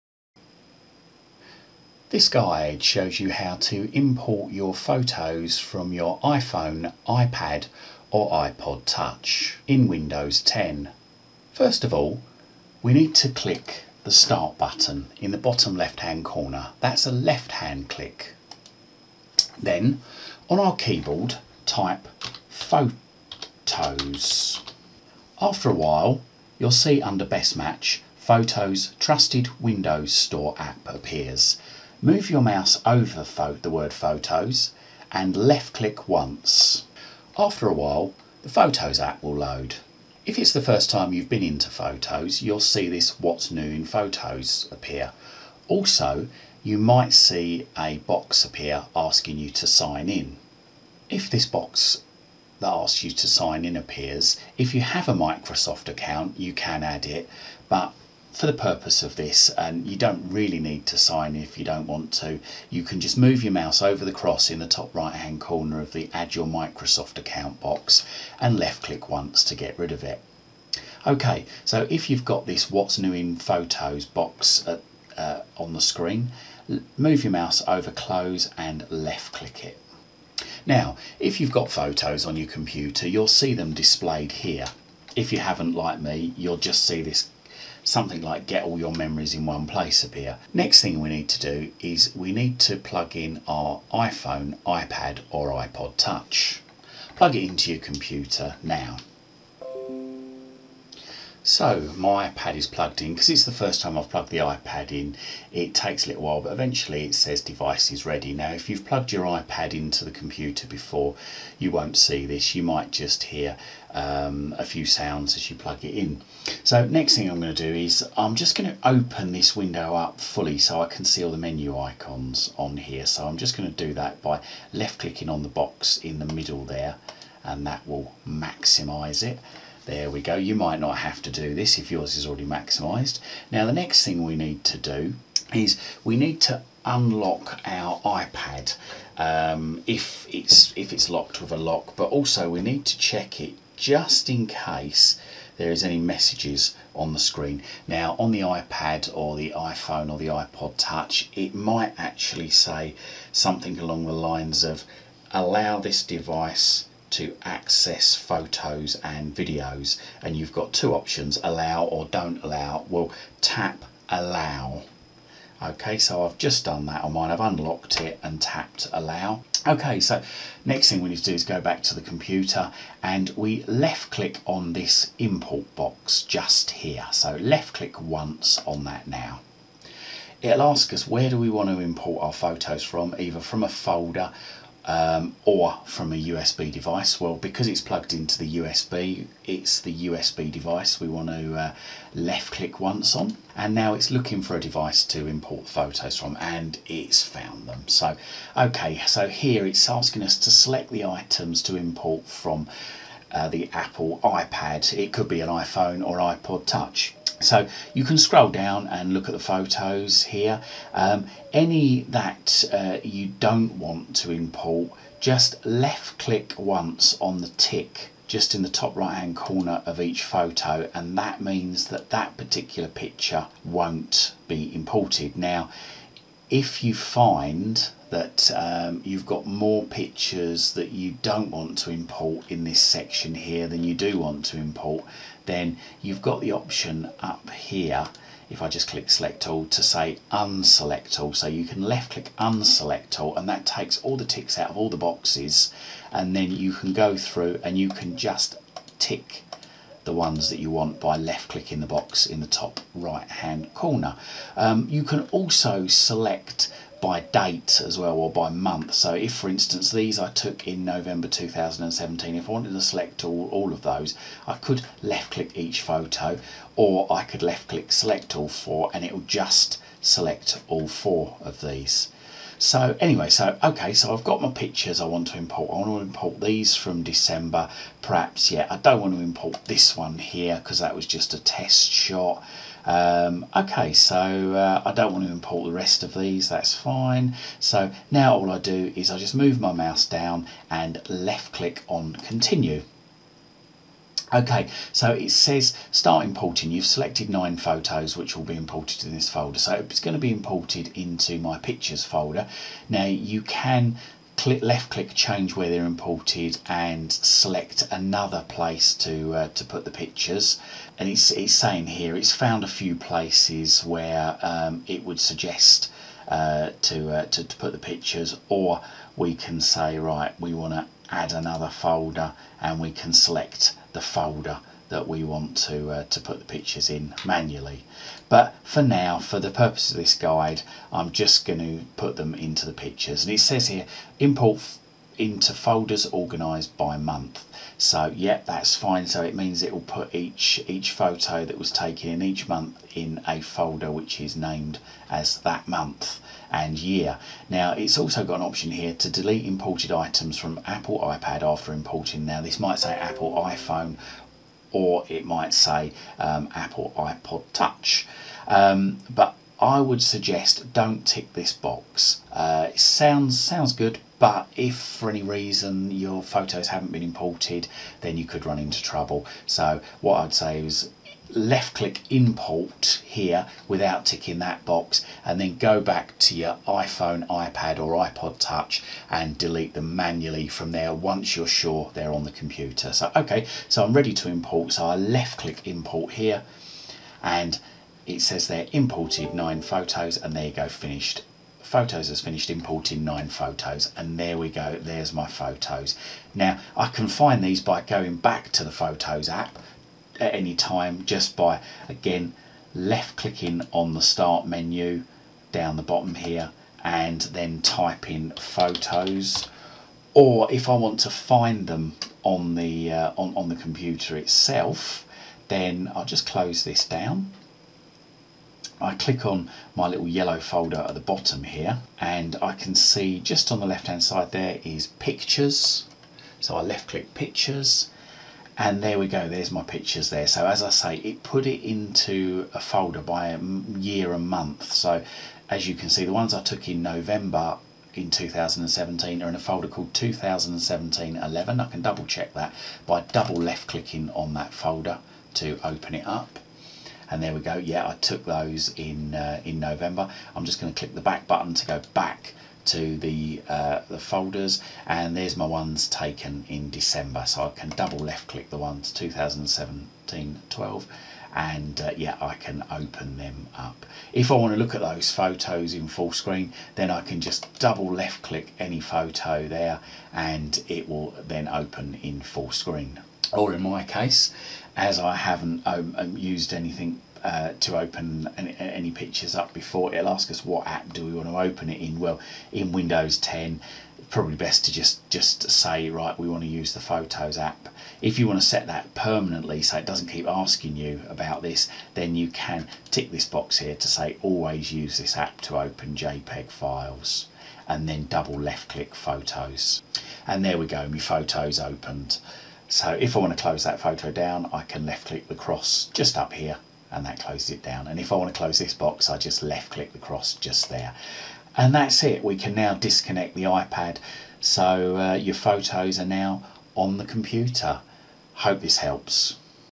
Import Photos to Win 10 from iPhone iPad or iPod Touch
Shows you how to import your photos into Windows 10 from an iPad iPhone or iPod Touch.